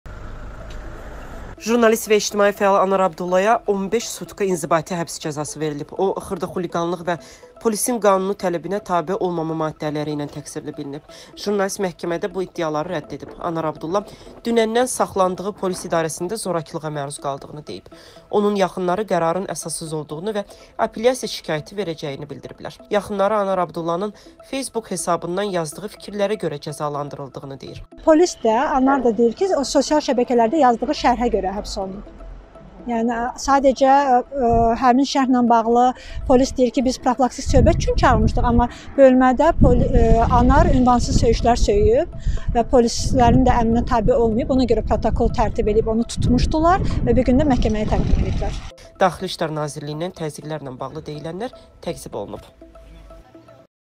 İlham Əliyevi tənqid edən jurnalistə polis bölməsində təzyiq olunduğu bildirilir
Jurnalist və ictimai fəal Anar Abdullaya 15 sutka inzibati həbs cəzası verilib.
O, xırda xuliqanlıq və polisin qanuni tələbinə tabe olmama maddələrilə təqsirli bilinib.
Jurnalist məhkəmədə bu iddiaları rədd edib.
Anar Abdulla dünəndən saxlandığı polis idarəsində zorakılığa məruz qaldığını deyib.
Onun yaxınları qərarın əsassız olduğunu və apelyasiya şikayəti veriləcəyini bildirib.
Yaxınları Anar Abdullanın “Facebook” hesabından yazdığı fikirlərə görə cəzalandırıldığını deyir.
Daxili İşlər Nazirliyindən təzyiqlərlə bağlı deyilənlər təkzib olunub.